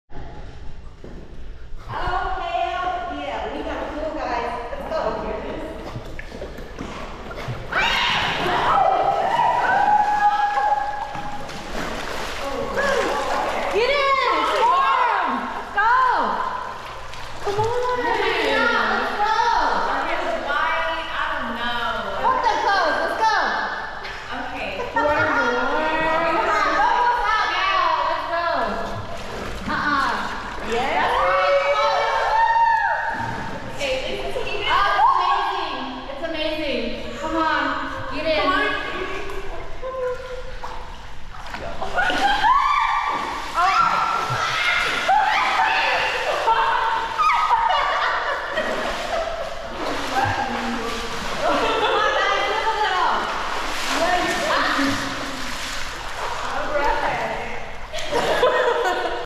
The Formal Pool Party Teaser. Funny girls
Four crazy ladies go in the pool fully clothed in their cocktail dresses.